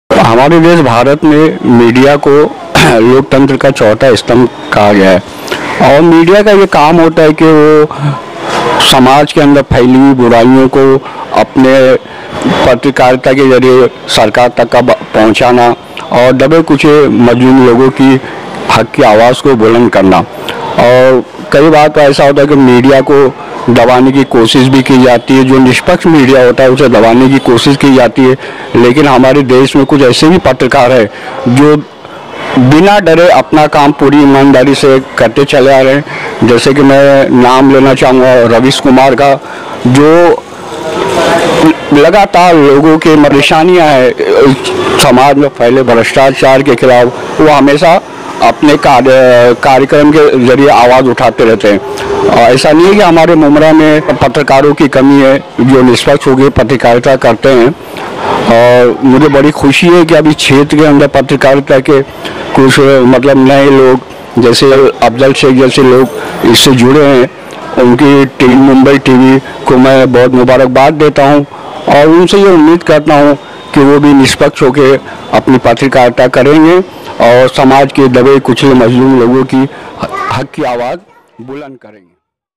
*Focus Kaynat Ke Sub Editor Mohiuddin shaikh Ne Di Mumbai Tv Ko Mubarakbaad*
*🎊🎉Mumbai tv Focus kaynat Ke Sub Editor Mohiuddin Shaikh ne di mubarakbaad dekhiye video*

Thanks for best wishes Mohiuddin Shaikh

Thanks For All My Supporter